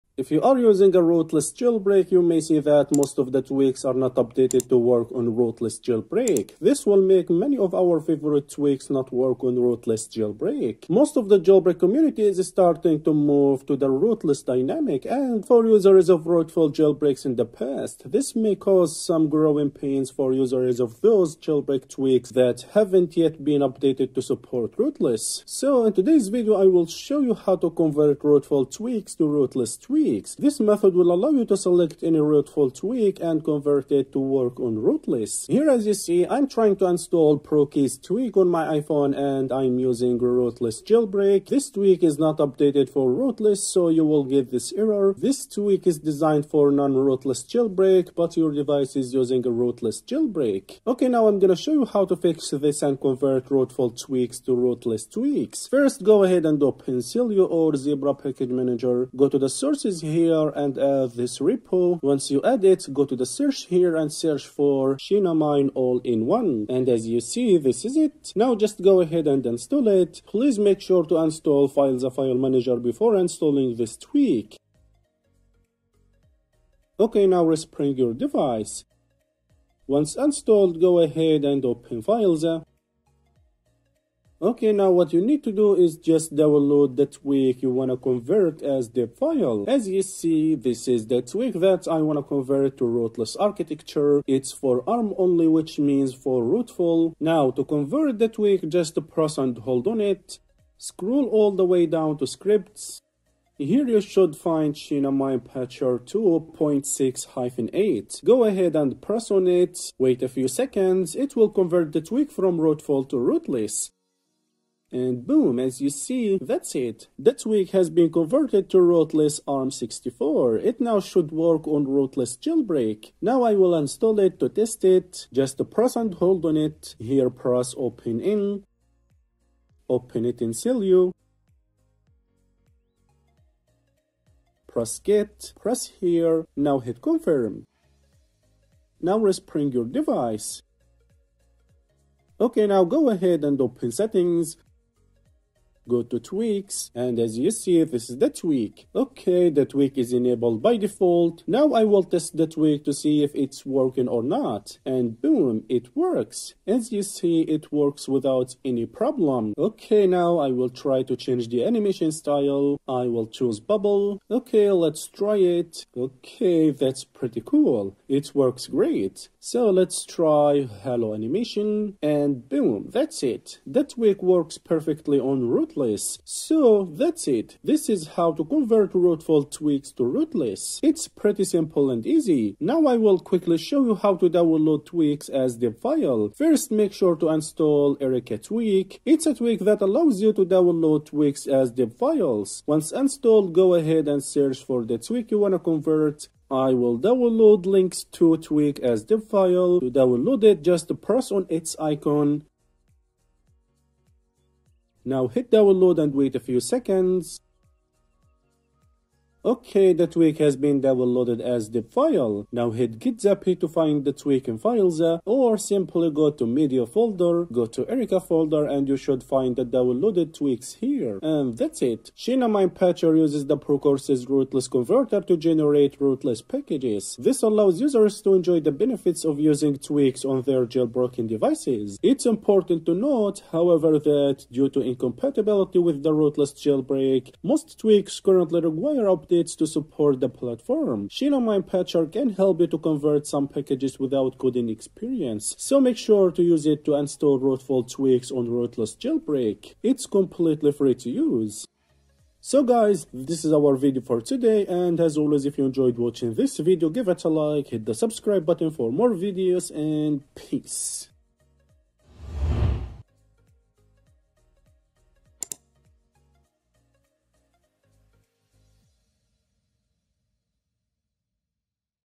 How to Convert Rootful Tweaks to Rootless | 2025 Easy Guide
If you are using a rootless jailbreak, you may see that most of the tweaks are not updated to work on rootless jailbreak, this will make many of our favorite tweaks not work on Rootless jailbreak. Most of the jailbreak community is starting to move to the rootless dynamic, and for users of rootful jailbreaks in the past, this may cause some growing pains for users of those jailbreak tweaks that haven’t yet been updated to support rootless. so in today's video I will show you how to convert Rootful tweaks to rootless tweaks.

✮═══ஜஜ🔆💜🔆ஜஜ═══✮
★═ஜ💠ஜ═✮

00:00 Intro & Rootless Jailbreak
00:36 Install Rootful tweak error
01:05 Xinam1ne AIO Patcher
01:37 Convert tweaks to rootless
02:05 Test the converted tweak
03:02 Download tweaks as deb
03:42 Final thoughts about Xinam1ne AIO
04:12 Thanks for watching & Peace